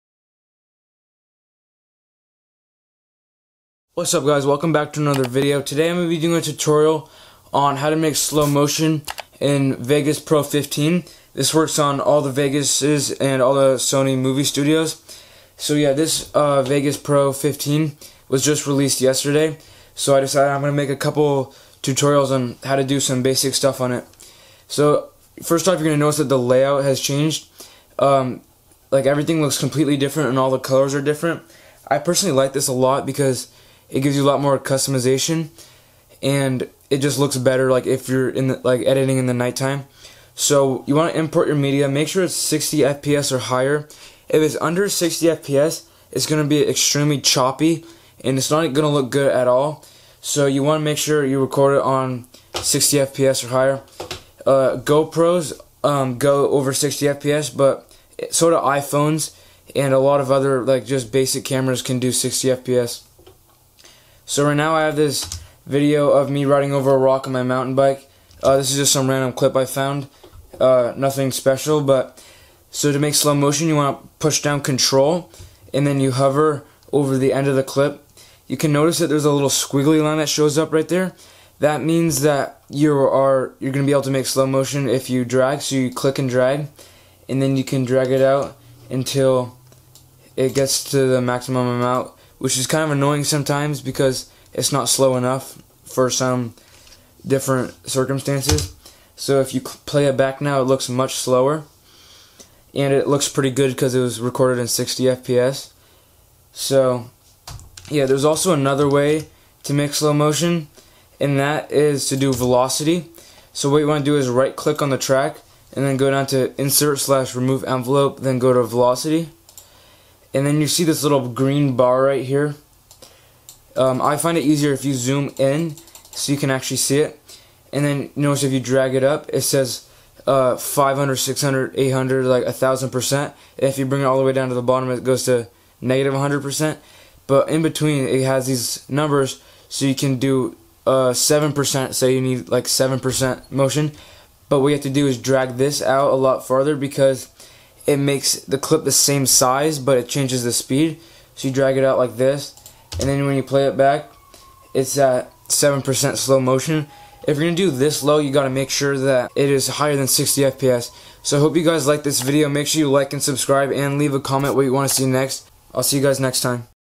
How to Make Slow Motion in Vegas Pro 15 (or any other vegas)! New | VegasMadeEasy
How to Make Slow Motion in Vegas Pro 15-14! New
In this video i go over the easiest ways to make slow motion in magix Vegas!